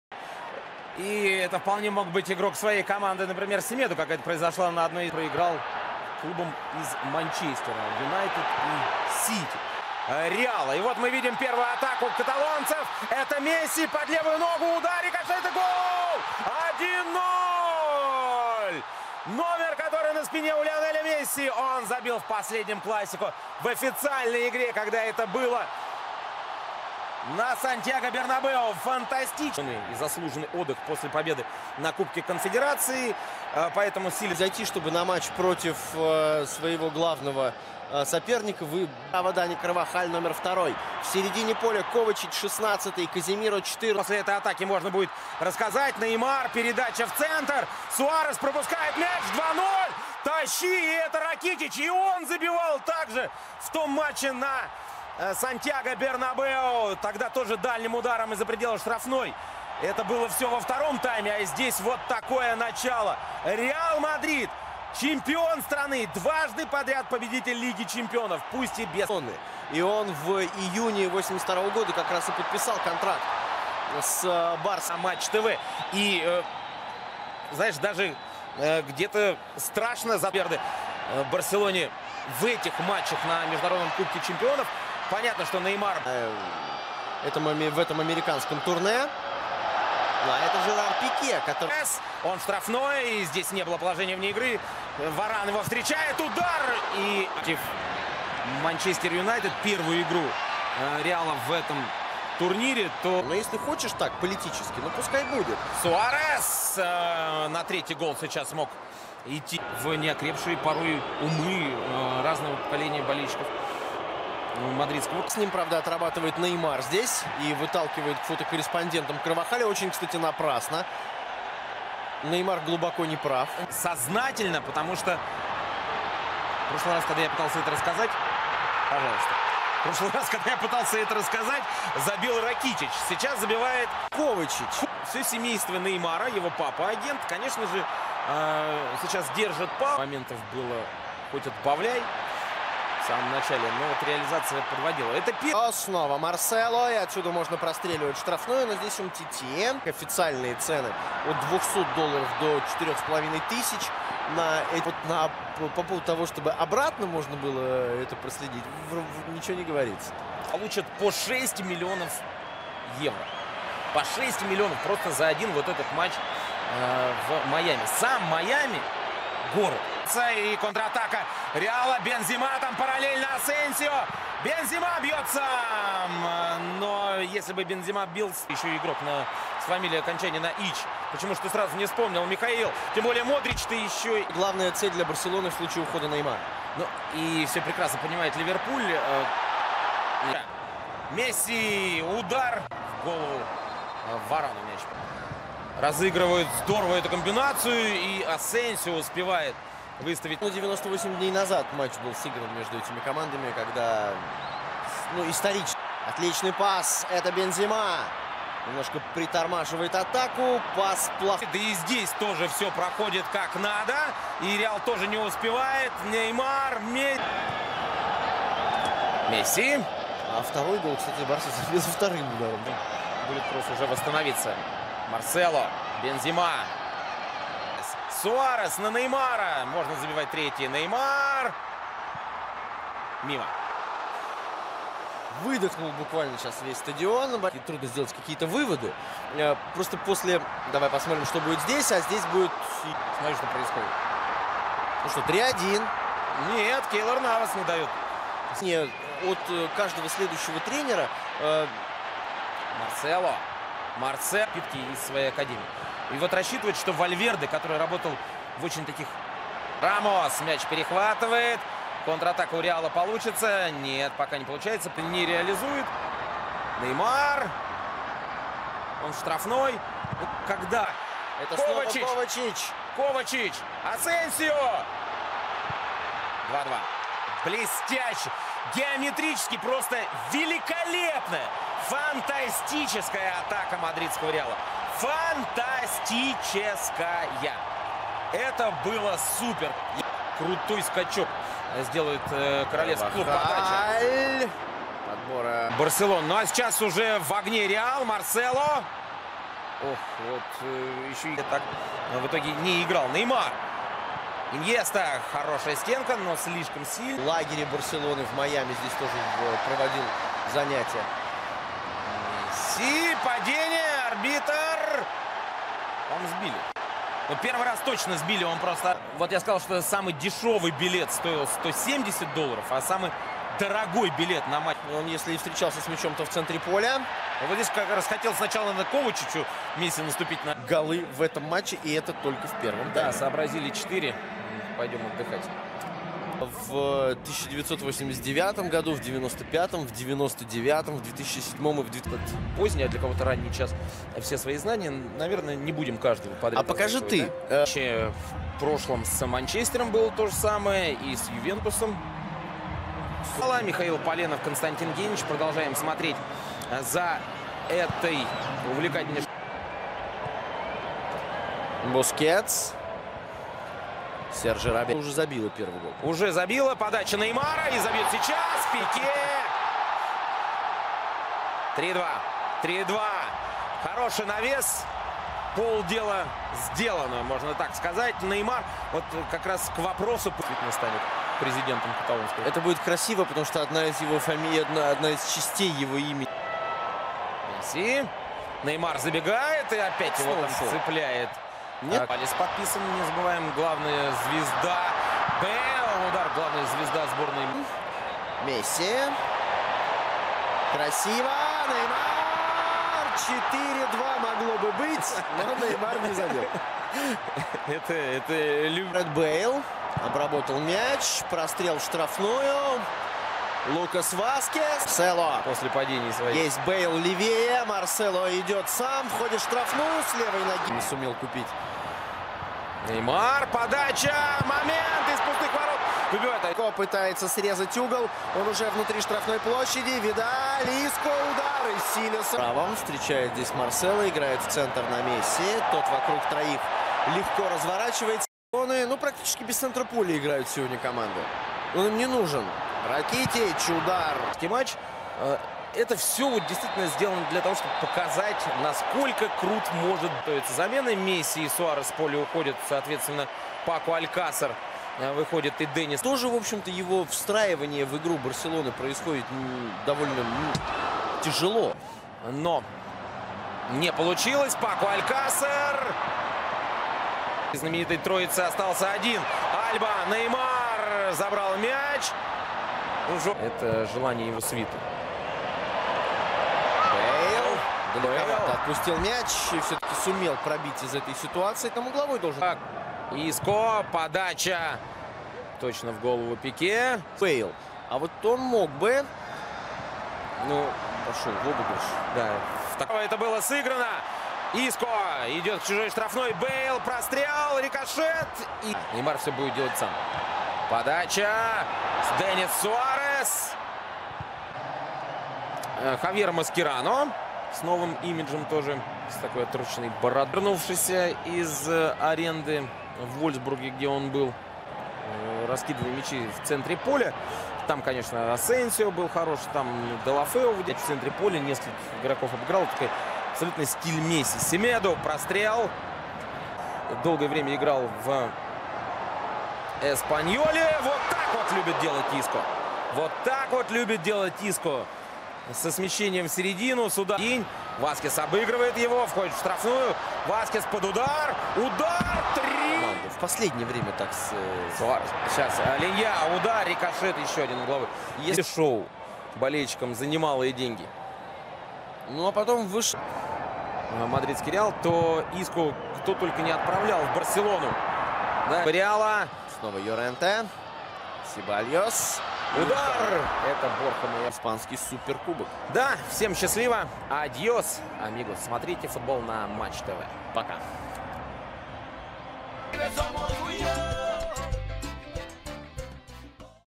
Real Madrid 2-3 Barcelona ● Messi (3') Rakitic (7') Piqué (50') And Kovacic (14') Asensio (36')
Real Madrid and Barcelona went head to head in the United States in 2017 with Barcelona winning 3–2 thanks to Gerard Pique’s winner.